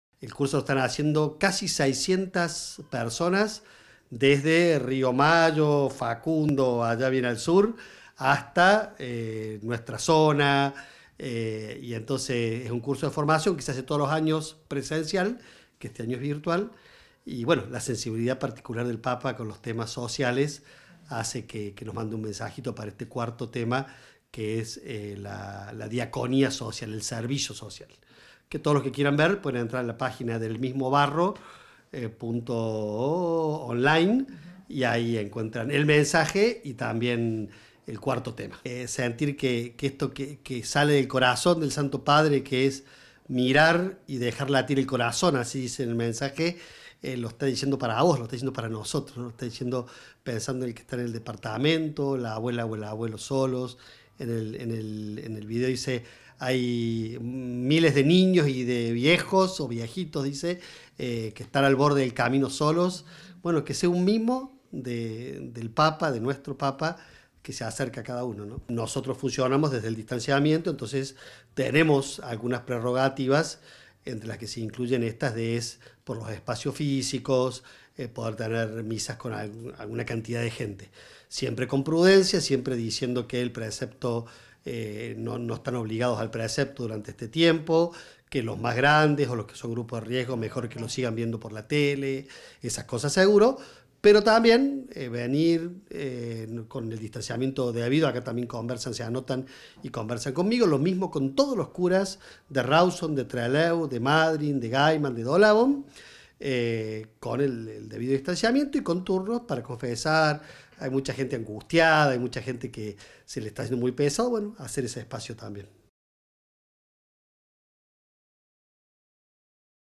Mensaje del Papa Francisco
El Papa Francisco se hizo presente en el curso de espiritualidad comunitaria de la Diócesis de Comodoro Rivadavia, con un video en el que comparte una reflexión, y asegura su cercanía y oración.